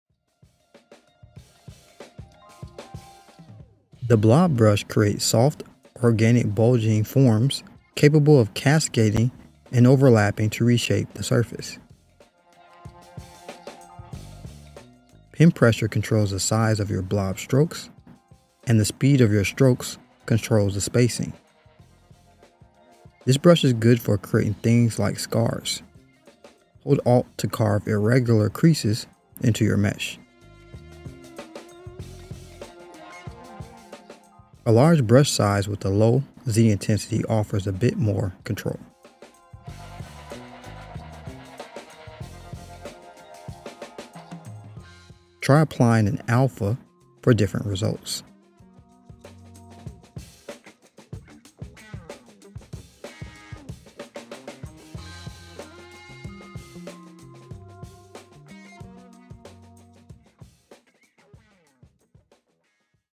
Blob Brush: ZBrush All Brushes
The Blob Brush creates  soft organic bulging forms capable cascading and overlapping to reshape the surface